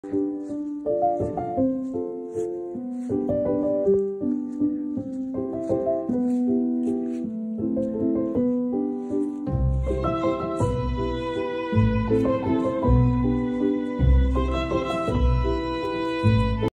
Gosling flying
I was carrying the goose to his food and he started flapping his wings and put his feet out as if he were flying 🥹

Gosling Baby goose baby goose rescue geese Birds Cute animals goose babygoose gosling cute animal videos cute baby animals Baby animals